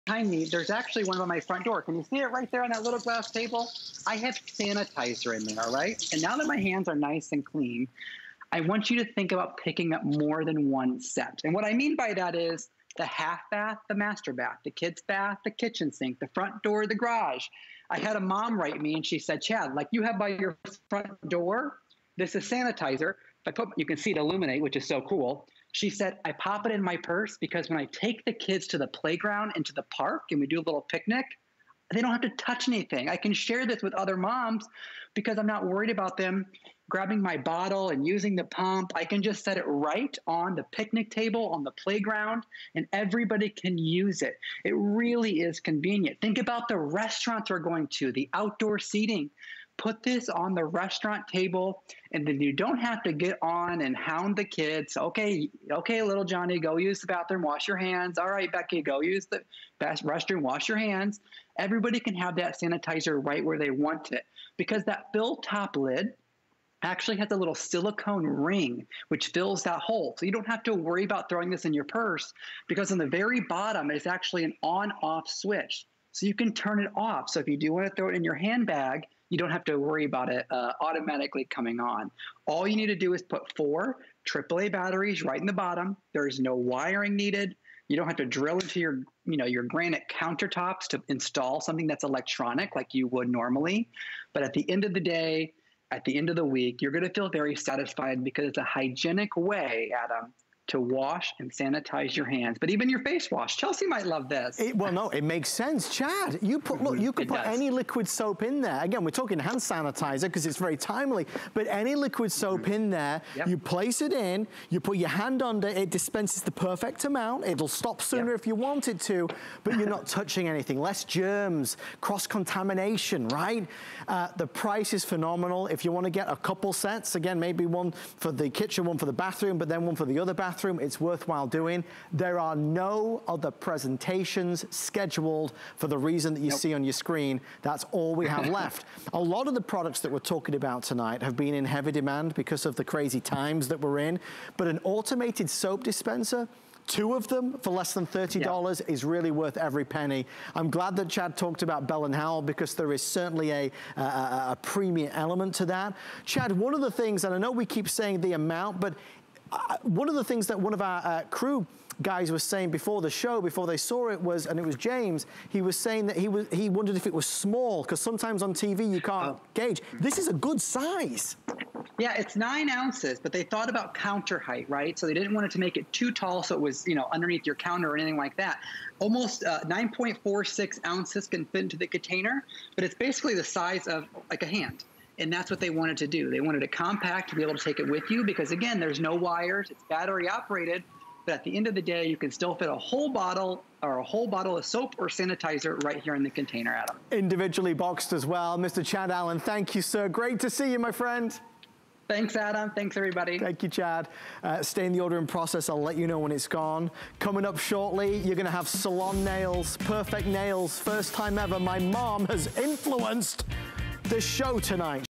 Bell + Howell Auto Sanitizer and Sonic Soap Dispenser 2p...
Bell + Howell Auto Sanitizer and Sonic Soap Dispenser 2pack
Dispense the perfect amount of soap or hand sanitizer fast and easy with this touchless innovative design. The dispenser is batteryoperated allowing you to put Sonic Soap anywhere you need it. Fill it with any of your favorite liquid soaps and watch it light up when dispensing.

    2 Sonic soap auto dispensers

    2 Instructions

    Requires 4 AAA batteries (not included).
    One LED light (installed).